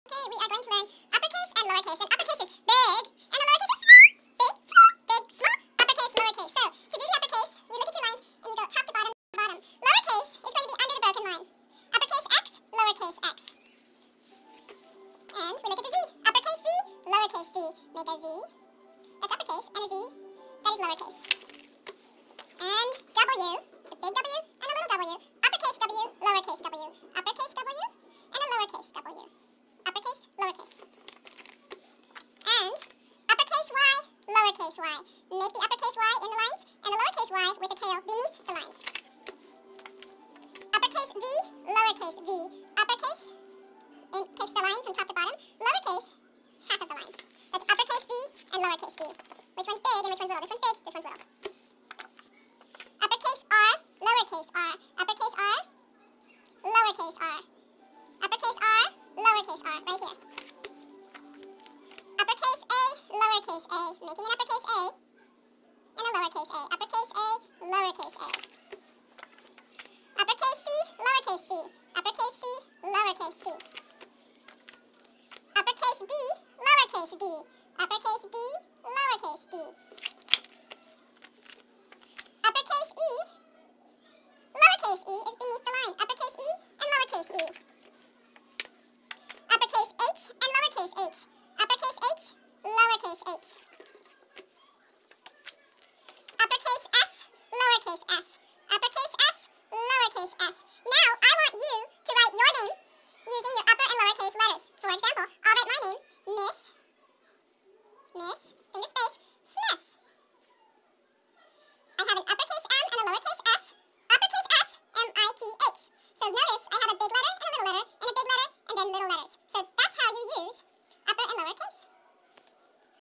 Uppercase and Lowercase Letters--Fast!!!
Uppercase and Lowercase writing practice and names using both